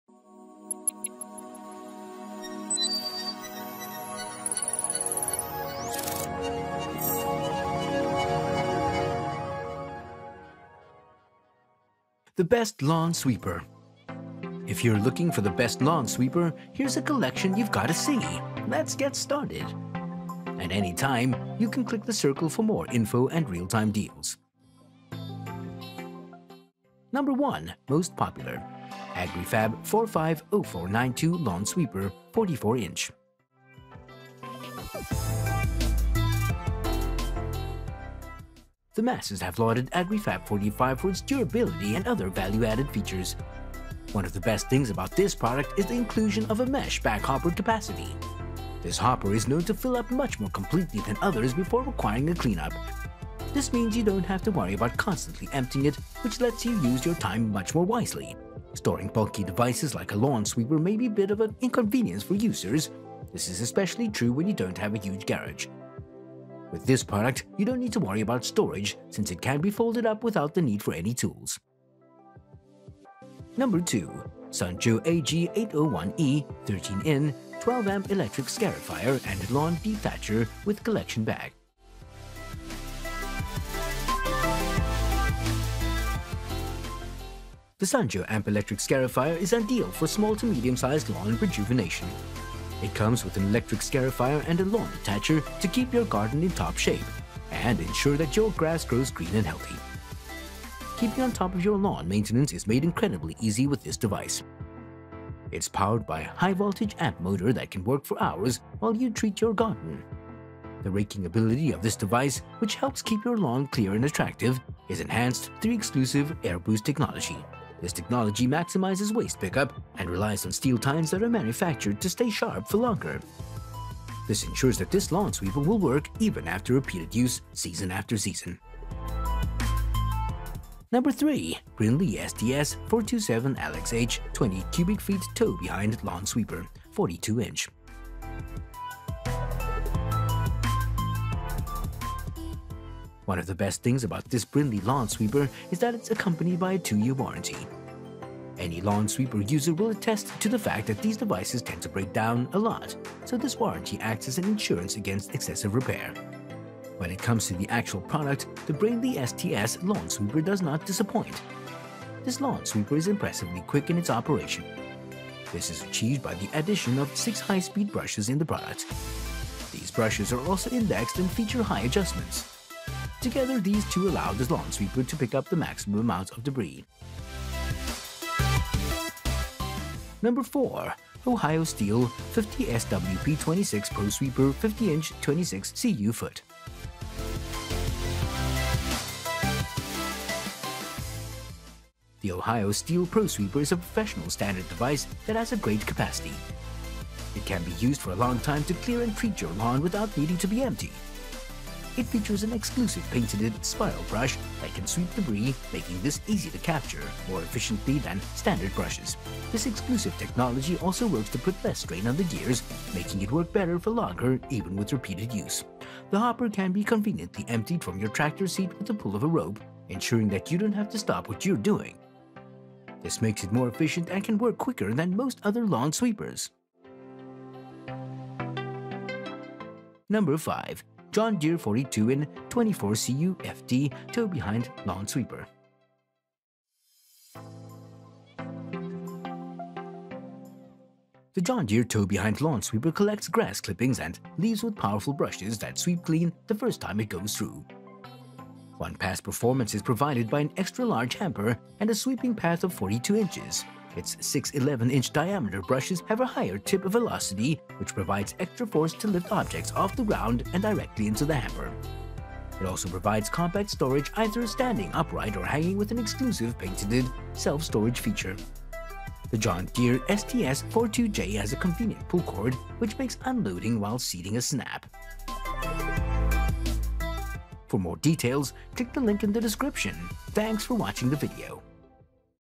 Top 5 Best Lawn Sweepers Review in 2024
Best Lawn Sweepers featured in this video-
0:26 NO.1. Agri-Fab 45-0492 Lawn Sweeper
1:13 NO.2. Sun Joe AJ801E 13 in. 12 Amp Electric Scarifier + Lawn Dethatcher w/Collection Bag
2:09 NO.3. Brinly STS-427LXH 20 Cubic Feet Tow Behind Lawn Sweeper
2:58 NO.4. Ohio Steel 50SWP26 Pro Sweeper,
3:47 NO.5. John Deere 42 in. 24 cu. ft. Tow-Behind Lawn Sweeper

What Is A Lawn Sweeper?
This machine can fundamentally supplant your rake, as it can clear up leaves, oak seeds, grass clippings, and different trash.

Lawn Sweeper Reviews, a site with the slogan "No More Raking!, offers genuine surveys of lawn sweepers, and clarifies that there are two kinds of lawn sweepers: Tow-behind and push.

What is the best Lawn Sweeper?
We strive to review as many products as we can, read realtime reviews, and made our own ranking. We welcome all your comments.